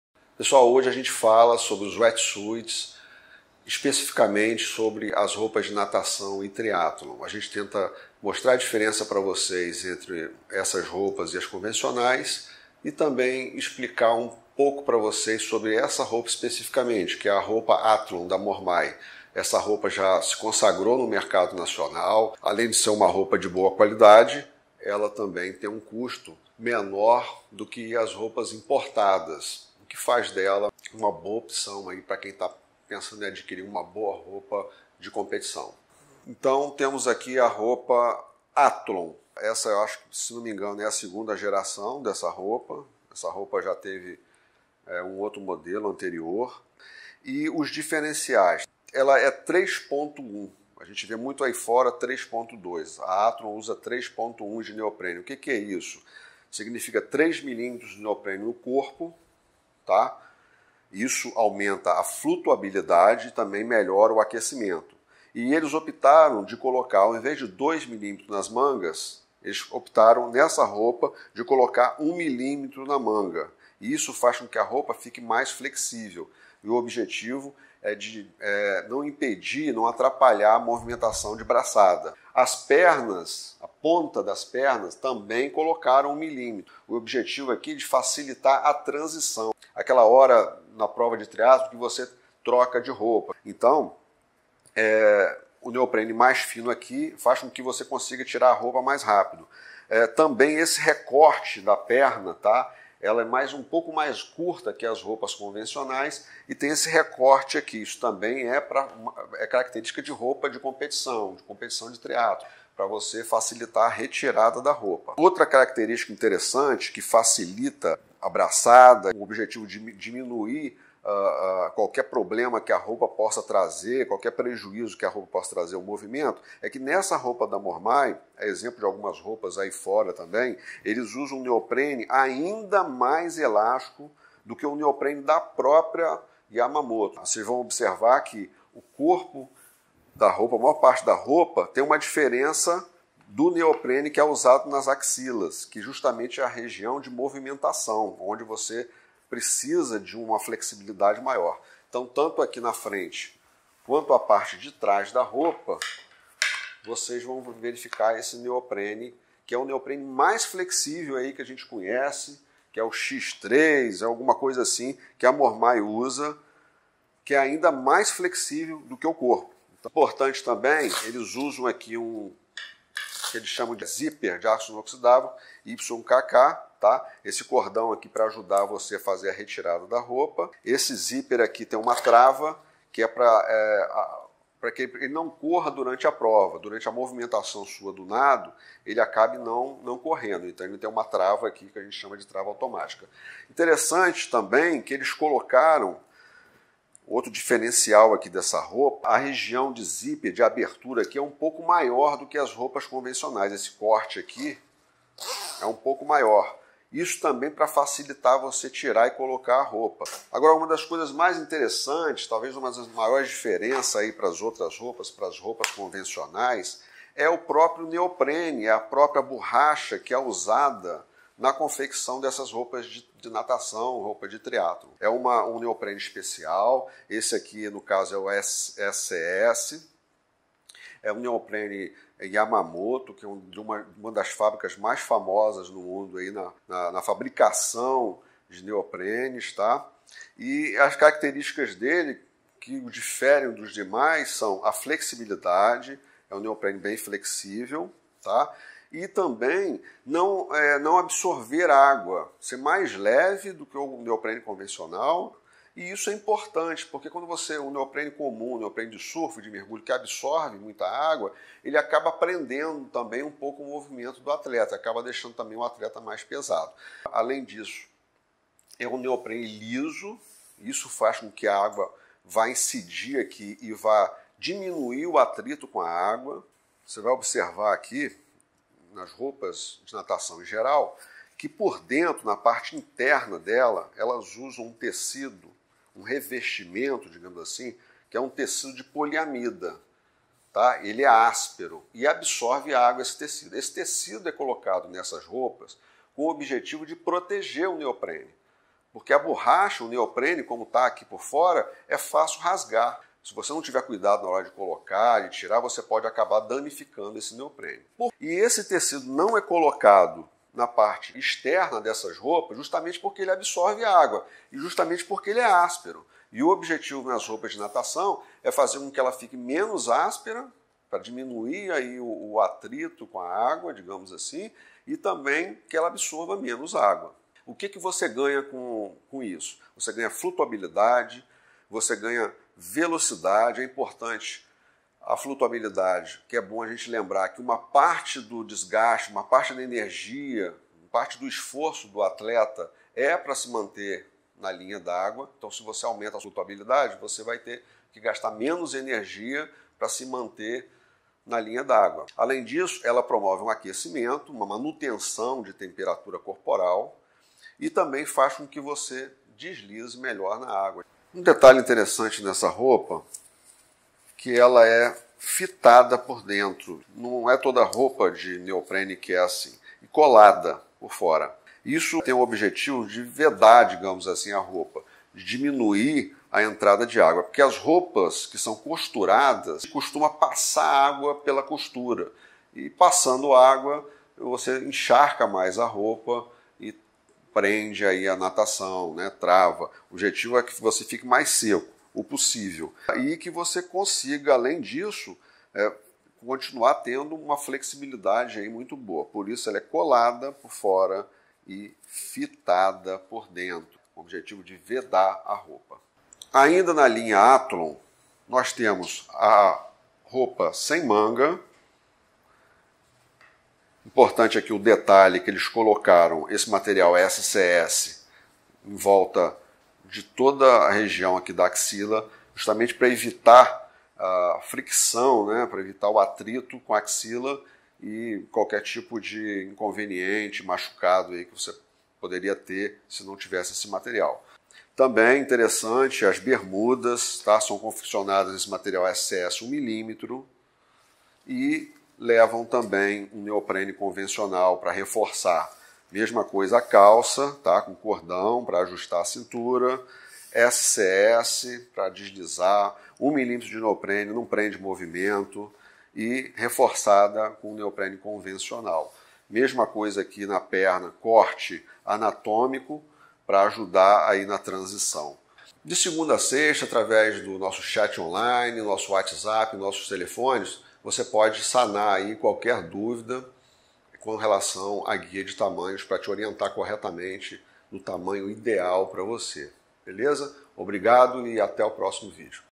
MELHOR ROUPA DE NATAÇÃO E TRIATLO (wetsuit , roupa de neoprene ou borracha )
As roupas de Natação e Triatlhon são diferentes, possuem uma neoprene especial (borracha) que é mais elástico que o convencional para não prender os movimentos.

Protegem do frio .Retém menos água assim fica mais leve e "seca" , são vedadas  o que faz com que retenham menos água que as roupas convencionais . Seu neoprene é liso  para "deslisarem" na água . Possuem várias características especias que lhe conferem flutuabilidade e velocidade . Sua matação nunca será a mesma depois de usar uma roupa desta . Além disso é aprovada pela FINA !

Se você gostou, entre em contato conosco para mais informações

👍 Para ficar por dentro de todas as novidades, curta nossa página no facebook